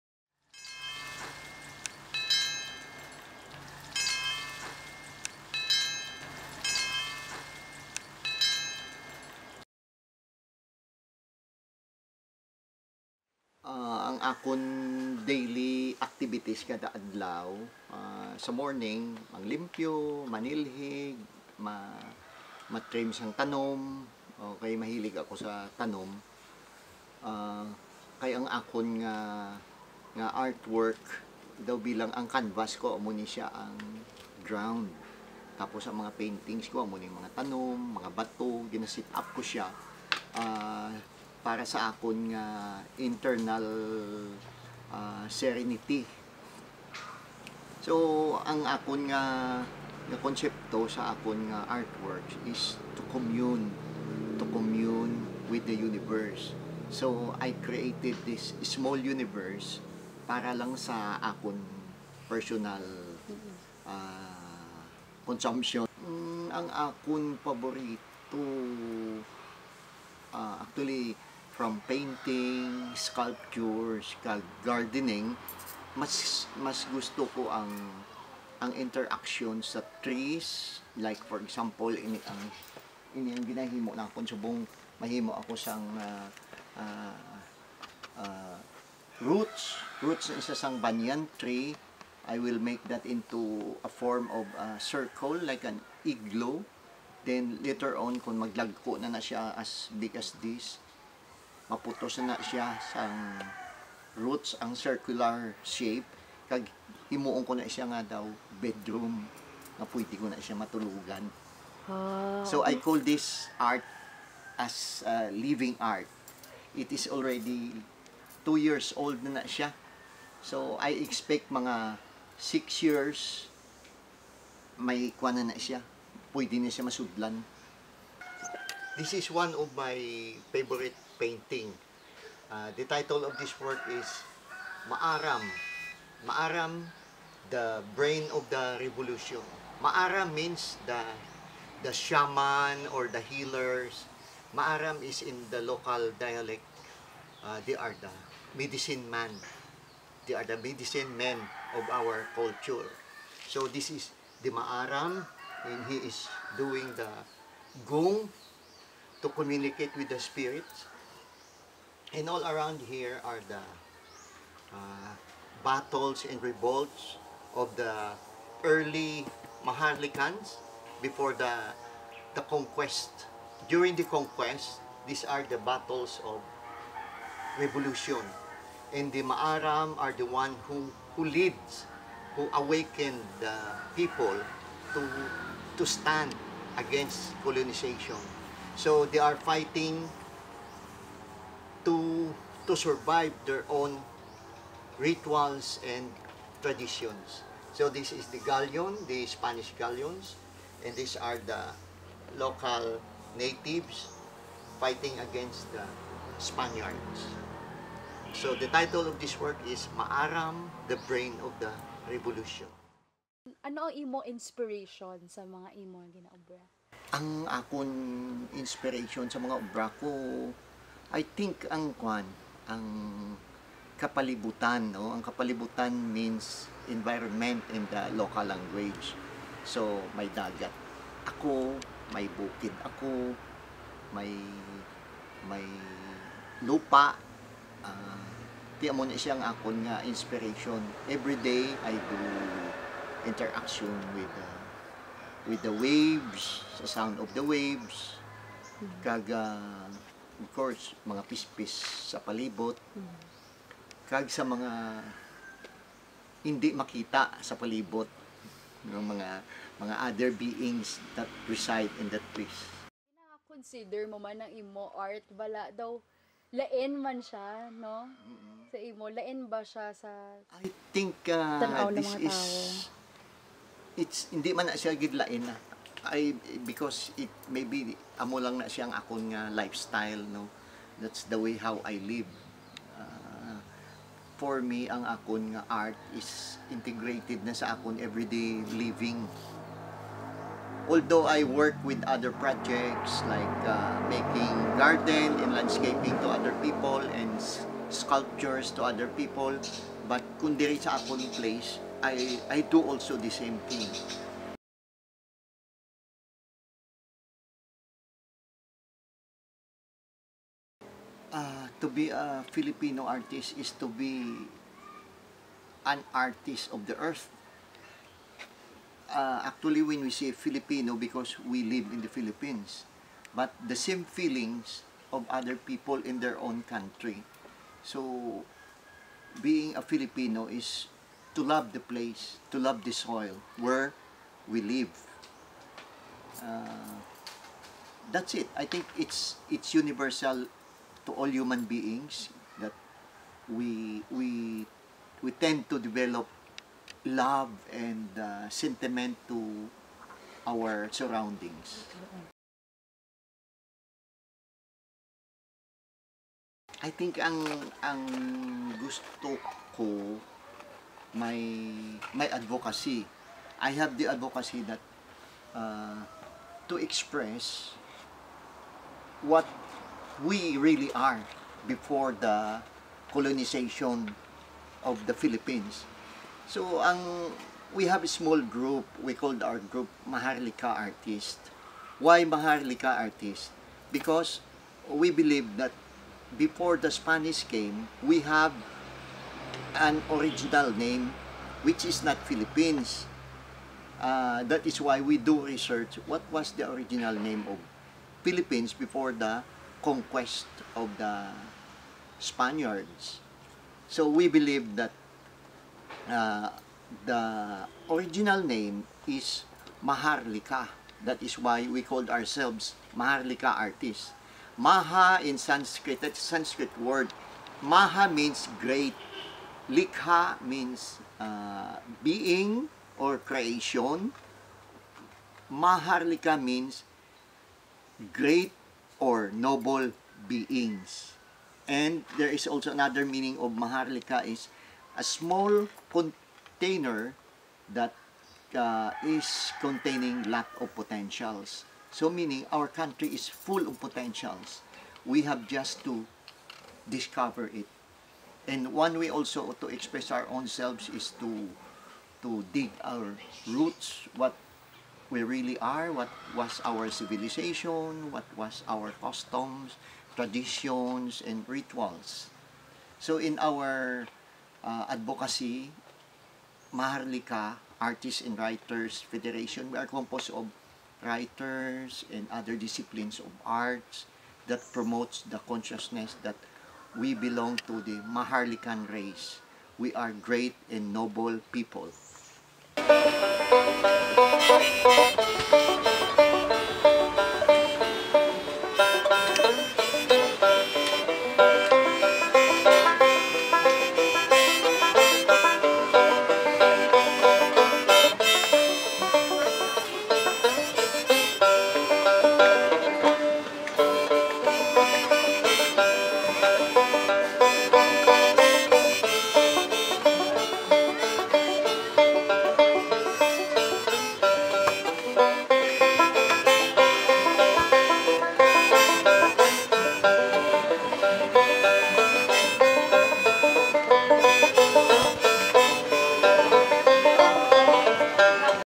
Gaginhawang Taliambong sang Kalibutan
Gaginhawang Taliambong sang Kalibutan (Breathing art of the Earth) is an in-depth interview with Ilonggo artist Momo Dalisay of San Joaquin, Lawigan. This is in completion of Understanding Philippine Arts and Culture course of UP Open University.